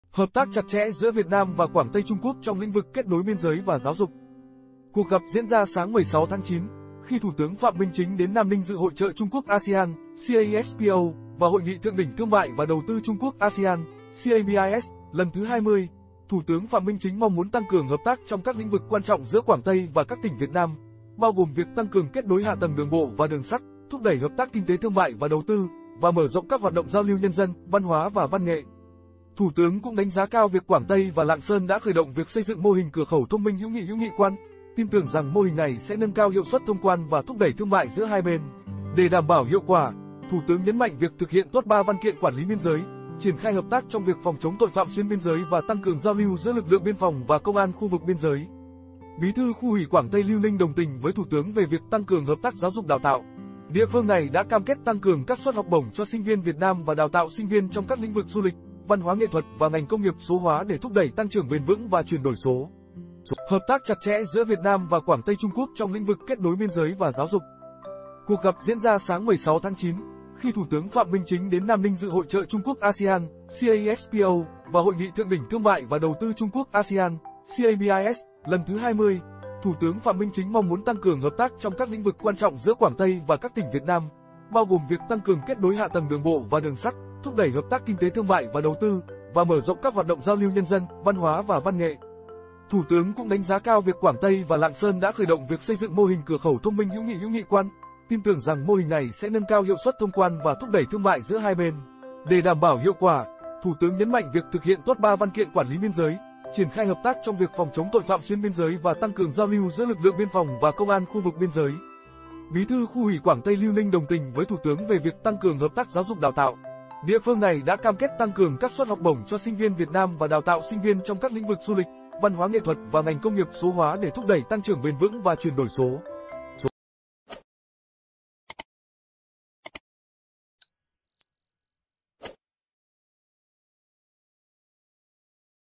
HỢP TÁC CHẶT CHẼ GIỮA VIỆT NAM VÀ QUẢNG TÂY TRUNG QUỐC TRONG LĨNH VỰC KẾT NỐI BIÊN GIỚI VÀ GIÁO DỤC
Cuộc gặp diễn ra sáng 16/9, khi Thủ tướng Phạm Minh Chính đến Nam Ninh dự Hội chợ Trung Quốc - ASEAN (CAEXPO) và Hội nghị Thượng đỉnh thương mại và đầu tư Trung Quốc - ASEAN (CABIS) lần thứ 20. Thủ tướng Phạm Minh Chính mong muốn tăng cường hợp tác trong các lĩnh vực quan trọng giữa Quảng Tây và các tỉnh Việt Nam, bao gồm việc tăng cường kết nối hạ tầng đường bộ và đường sắt, thúc đẩy hợp tác kinh tế - thương mại và đầu tư, và mở rộng các hoạt động giao lưu nhân dân, văn hóa và văn nghệ.